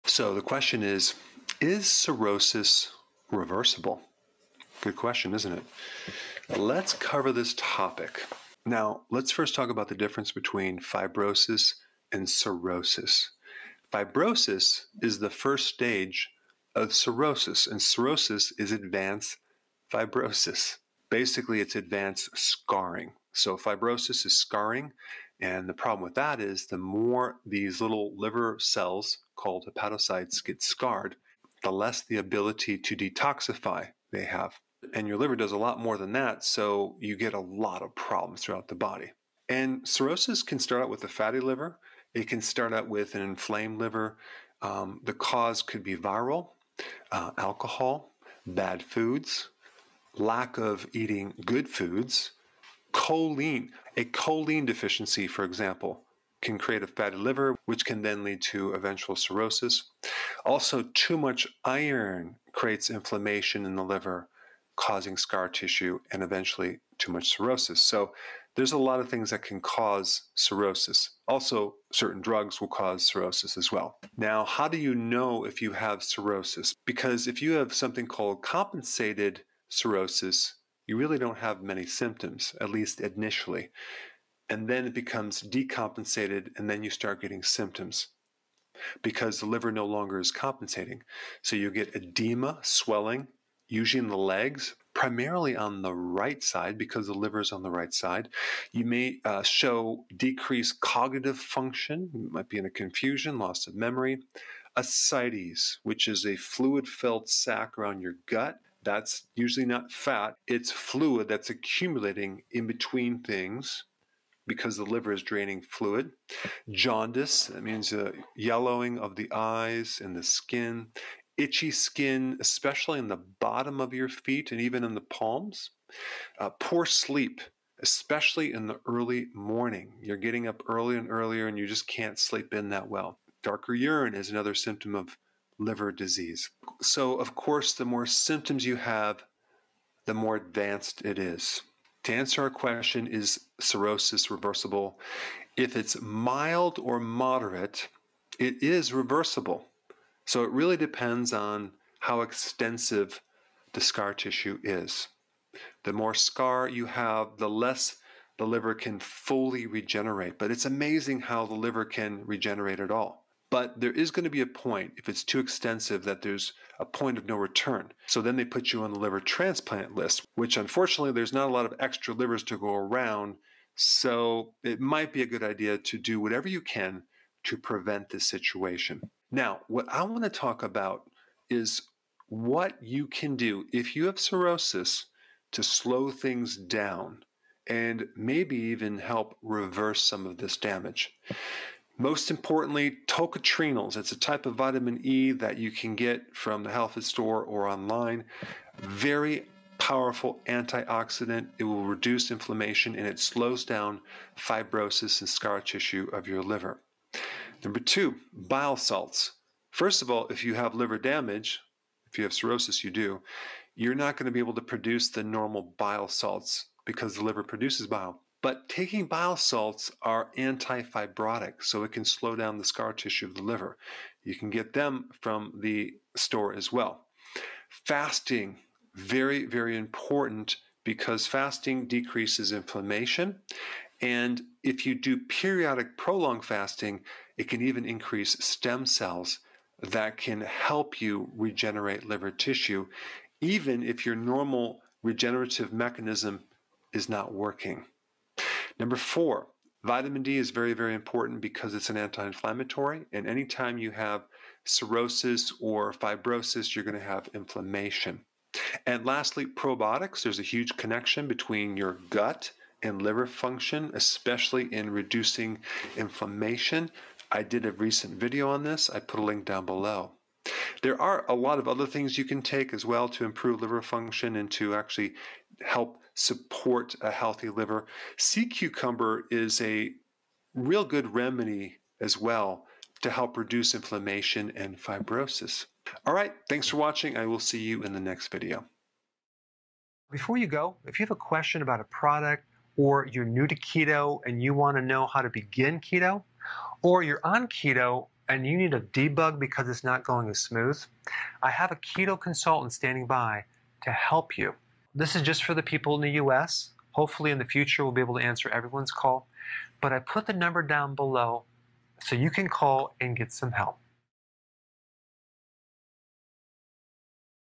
Is Liver Cirrhosis Reversible? - Fibrosis vs. Cirrhosis – Dr.Berg
What can be done about liver cirrhosis? Here’s what you need to know.

Timestamps
0:00 Is liver cirrhosis reversible?
0:10 Fibrosis vs. cirrhosis
0:39 How does fibrosis start?
1:14 How do you know if you have cirrhosis?
2:22 Can you reverse liver cirrhosis?
3:00 Slowing liver cirrhosis
4:50 Need keto consulting?

In this video, we’re going to talk about liver cirrhosis and whether or it is reversible.

First, let’s talk about the differences between fibrosis and cirrhosis. Fibrosis is the first stage of cirrhosis, while cirrhosis is an advanced version of fibrosis. Essentially, cirrhosis is advanced scarring in the liver.

The more scarring in your liver, the less your liver has the ability to detoxify your body. In turn, you may have a build-up of toxins in your body if you have liver fibrosis or cirrhosis.

Liver fibrosis can start out with a fatty liver, liver inflammation, viral infections, alcohol consumption, a poor diet, low choline levels, too much iron, and certain drugs.

How do you know if you have cirrhosis? In many cases, the symptoms are non-existent. However, the signs of liver cirrhosis may include:
• Edema
• Low cognitive function
• Ascites
• Jaundice
• Itchiness
• Poor sleep
• Dark urine

The more symptoms you have, the more advanced it is.

So, back to the question, can you reverse liver cirrhosis? The answer is: it depends. If you have mild fibrosis or cirrhosis, it may be possible to reverse some of the damage. However, if you have severe liver cirrhosis, the problem may not be reversible.

The most important thing is to do whatever you can to avoid liver cirrhosis in the first place.

Here are the best ways to support a healthy liver:
• Tocotrienols
• Bile salts
• Fasting
• Vitamin D
• Probiotics

I hope this helped clear up how to support a healthy liver and avoid cirrhosis.